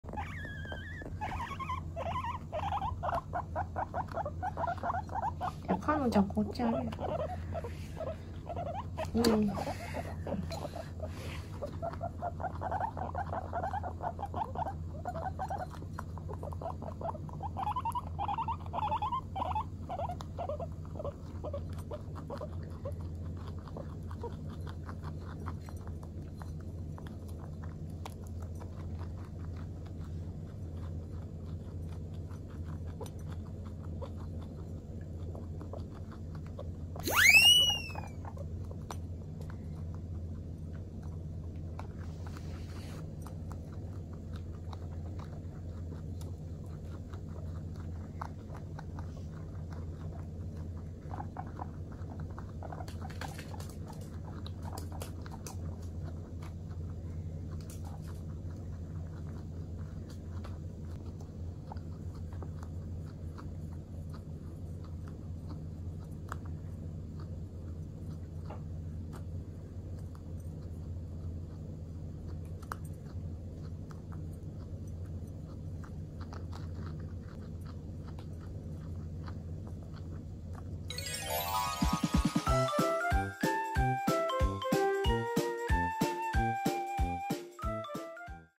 【モルモット】2人でダブルASMR♪ 咀嚼音！
歌音のASMRも見たいとリクエスト来ましたので、2人でチャレンジしてみました✨😍✨

けど歌音女王様は隣のティンクのが気になるようで、それちょうだいとしつこくティンクに迫ります！！

ティンクは必死に鳴いて、嫌だと阻止していました😂💕

そんなところから動画スタートです✨😆✊🏻

是非音量を上げて聴いてみてください🤩

うちには2匹のモルモットがいます。

歌音（かのん） 長毛 ♀ 2018/1/1生まれ

性格はマイペースで、女王気質👑

ティンク     短毛 ♀ 2018/2/15生まれ

性格は好奇心旺盛のおてんば✨👍🏻

よければそちらも覗いてみてください💕✨